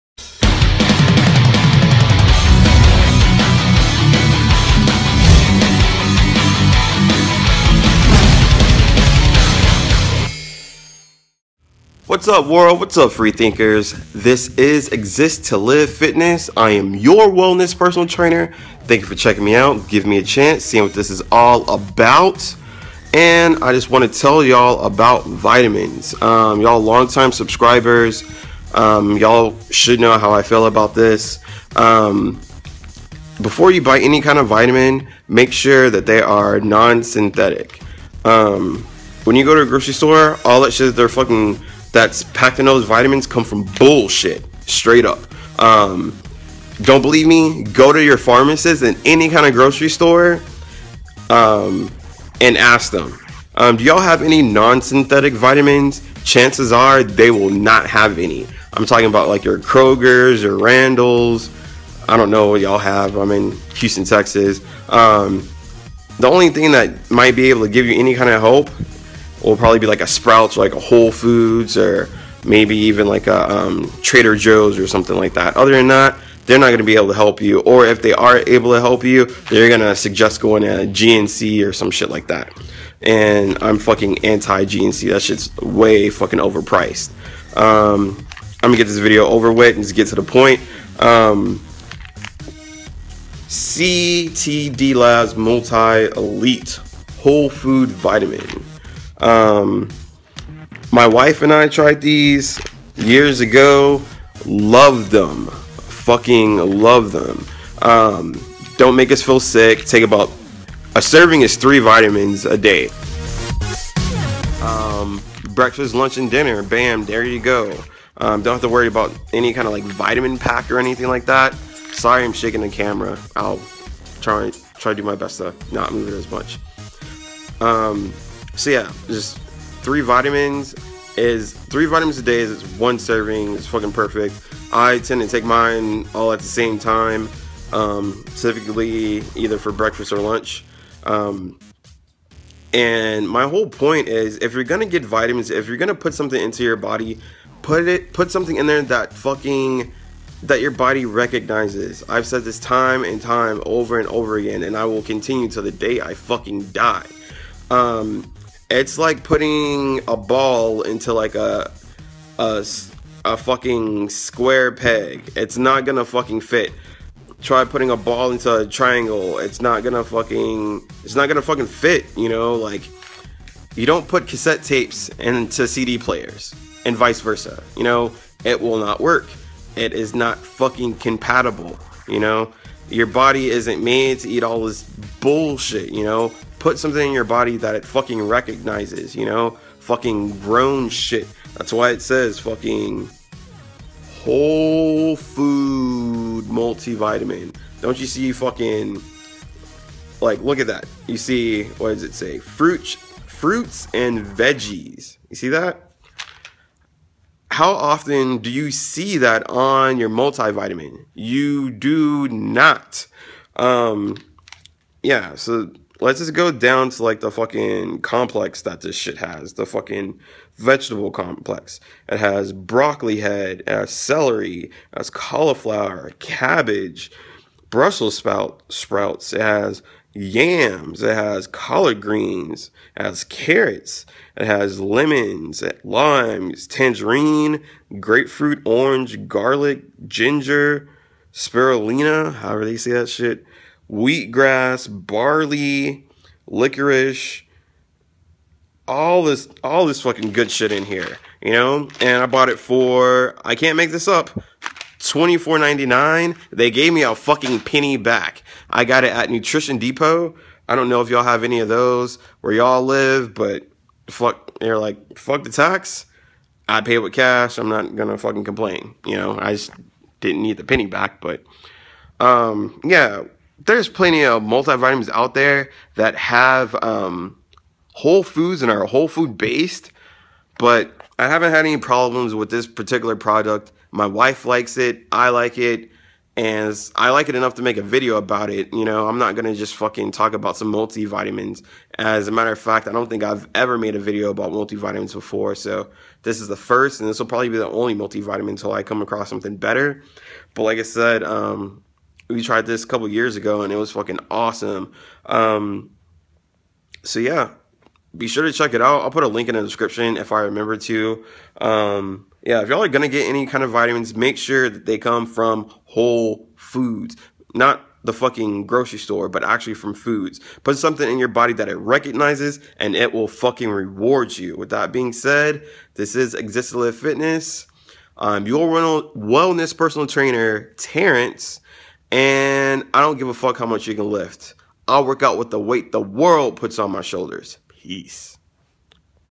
Ctd Labs Multi Elite Whole Food Vitamin Review
CTD Labs Multi Elite vitamins.  Research and invest in yourself, health is wealth. Are you taking synthetic vitamins? Instead of whole food based vitamins? Give your body what it wants and it will pay you back graciously.